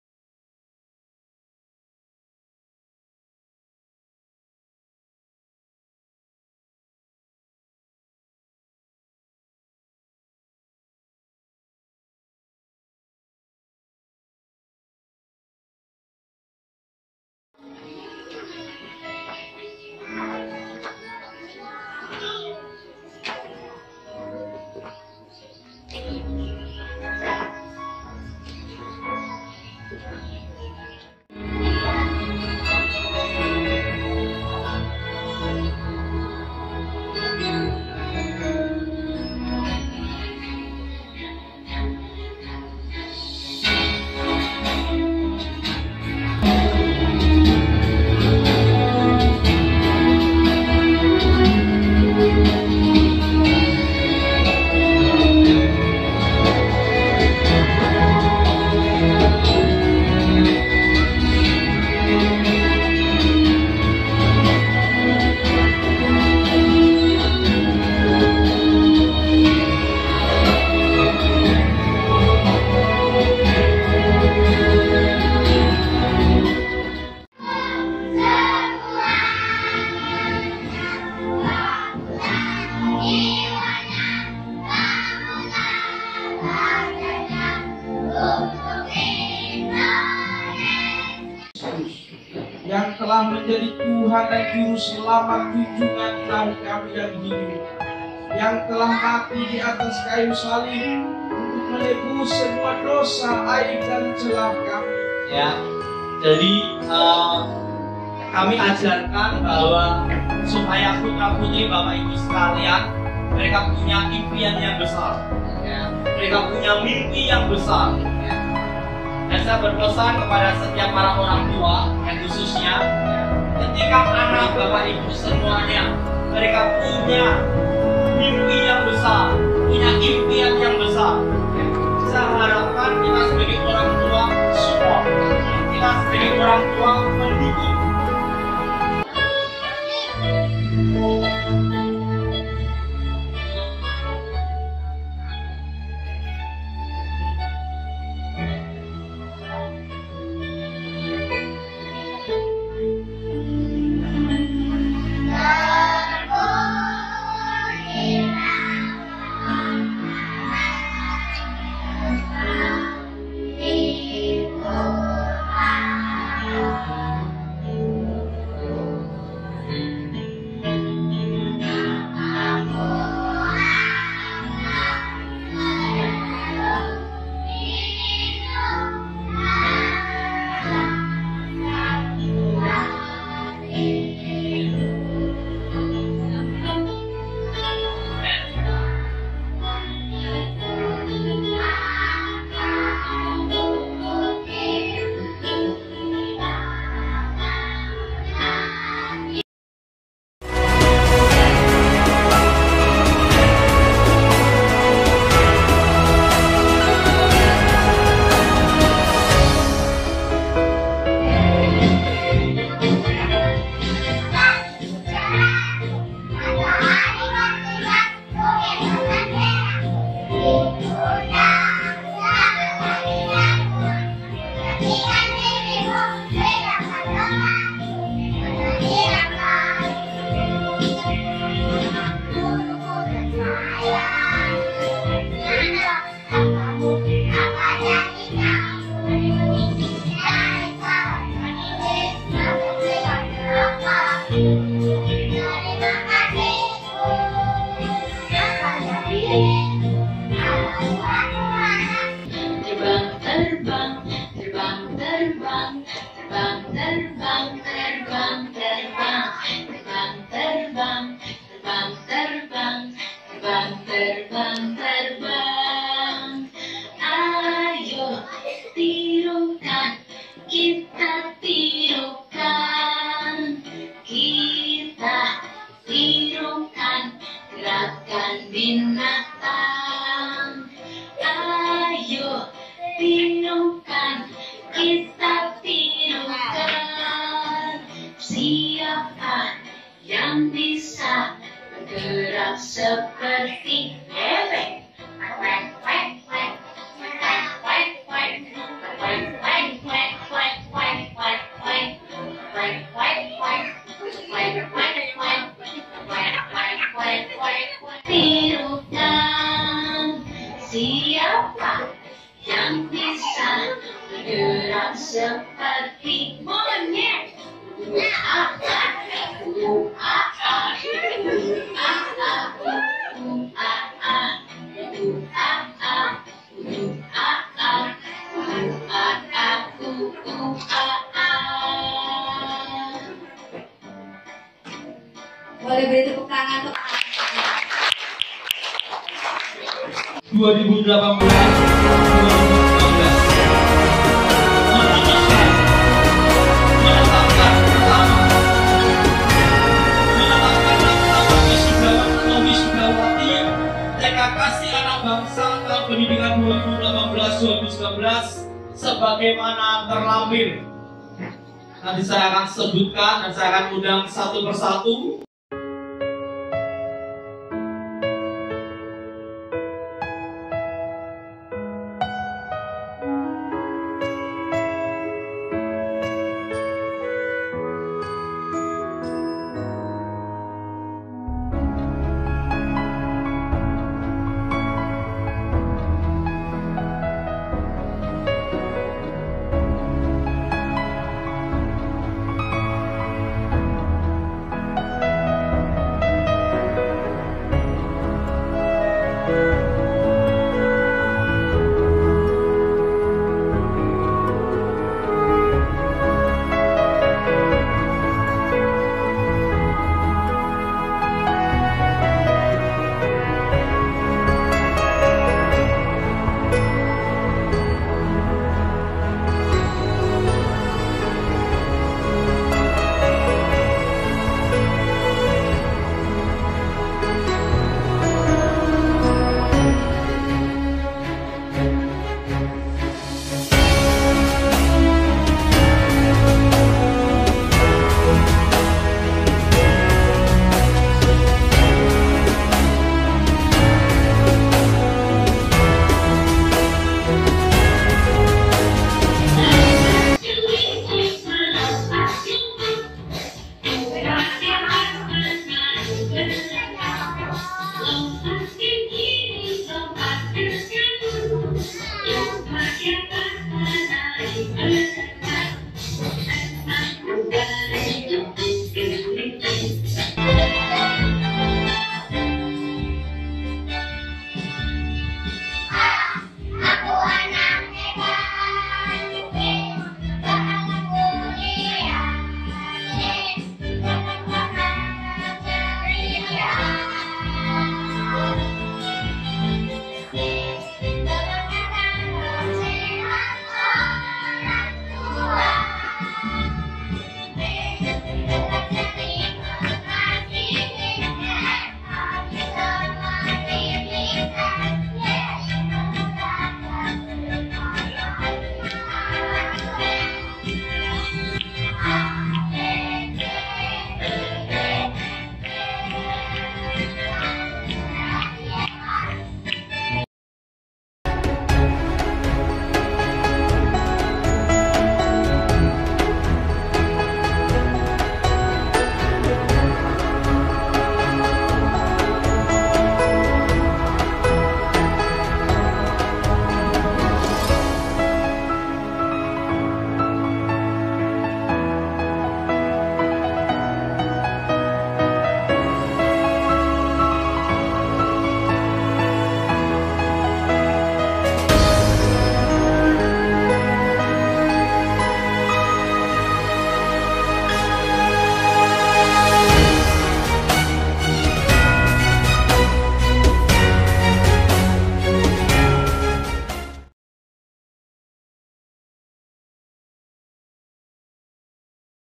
GRADUATION 2nd GENERATION 2019
Graduation TK Kasih Anak Bangsa angkatan kedua ini merupakan acara puncak semester 2 yang menjadi program tahunan dari sekolah TK Kasih Anak Bangsa

Tahun ini TK Kasih Anak Bangsa sudah meluluskan 32 Anak termasuk dari beberapa sekolah cabang. Kai ini kami mengangkat tema Dare to Dream.